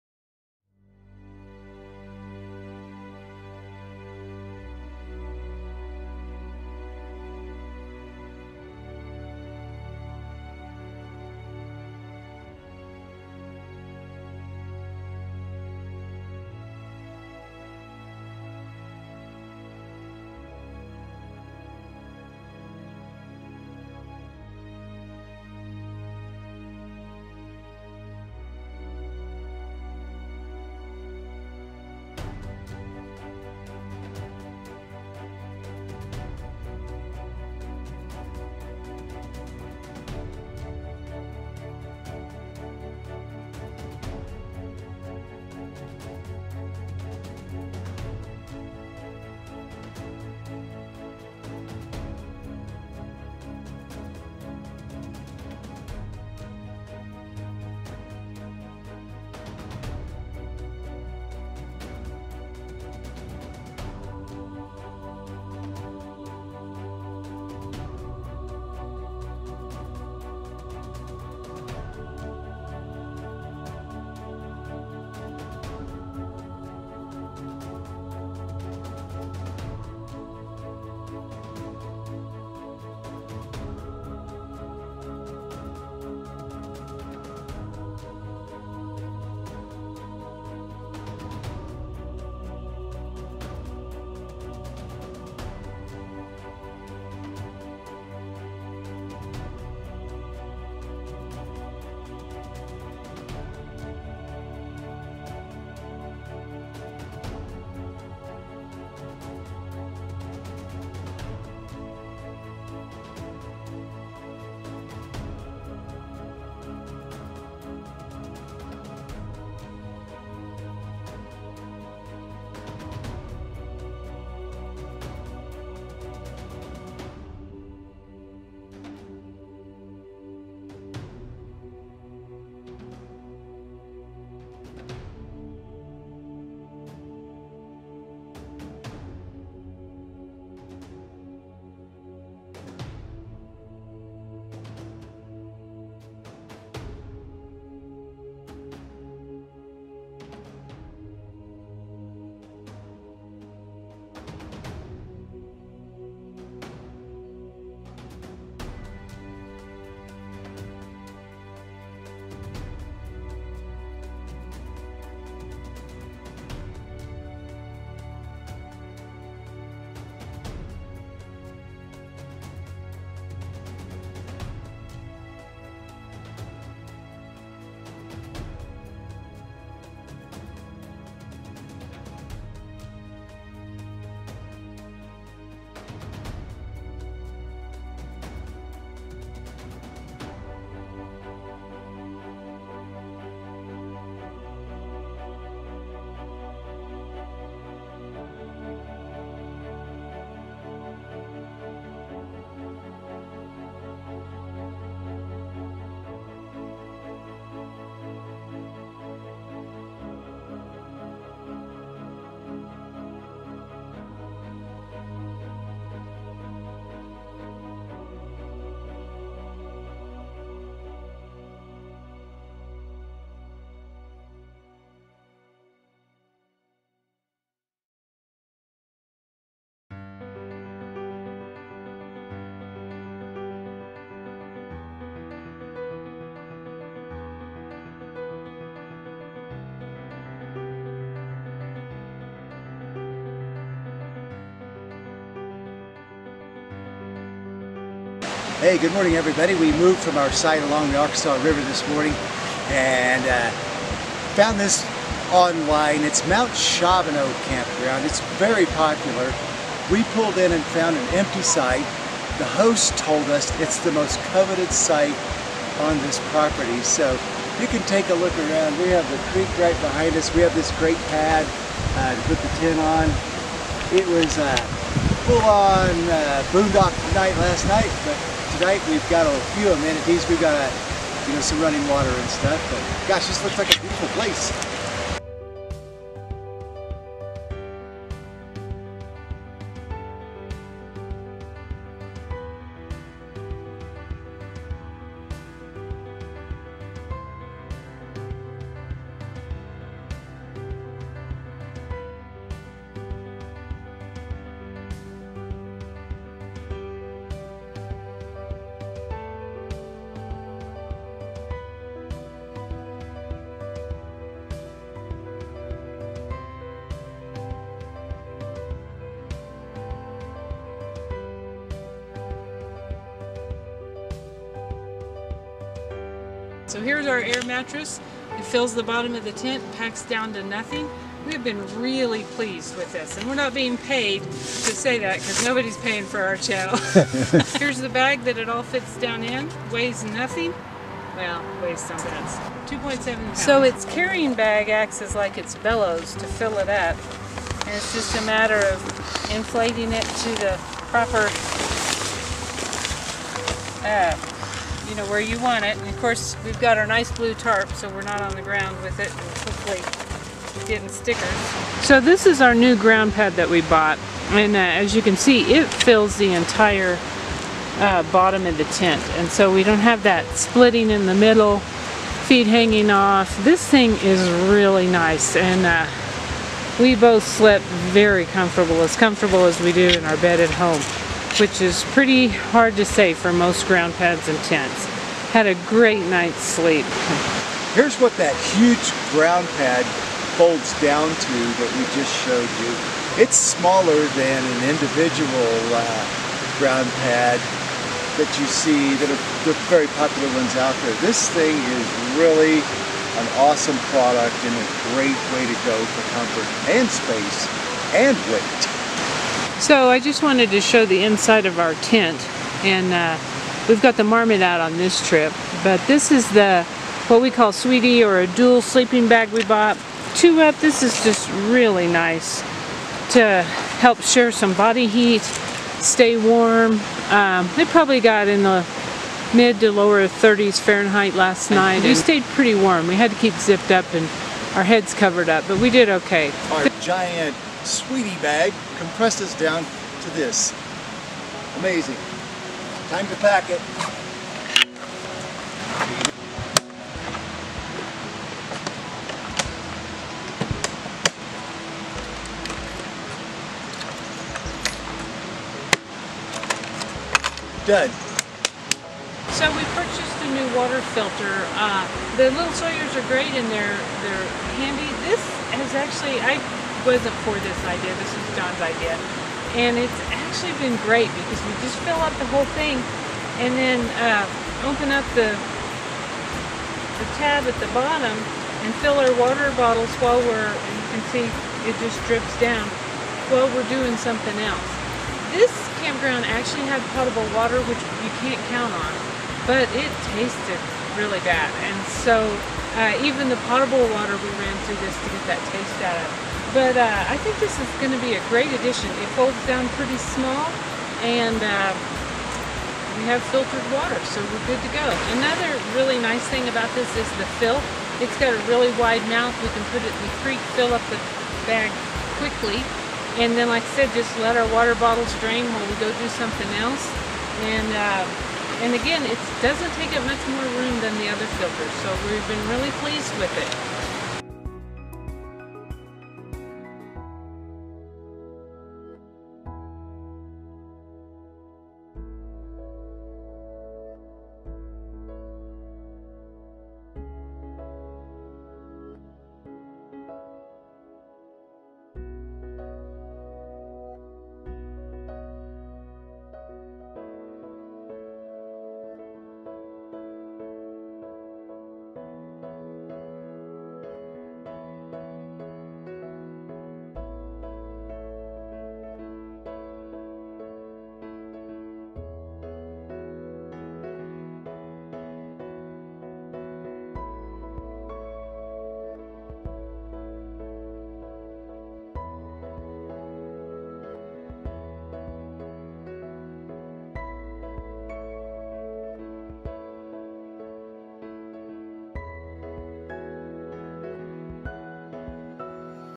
Arkansas River and Angel of Shavano motorcycle camping. Music by Bensound.com
This trip we spent the first night along the Arkansas river. The following day we camped at the Angel of Shavano campground.
In this video we also show two new pieces of camping equipment that we recently purchased. Come along for the ride and discover the joy of motorcycle camping!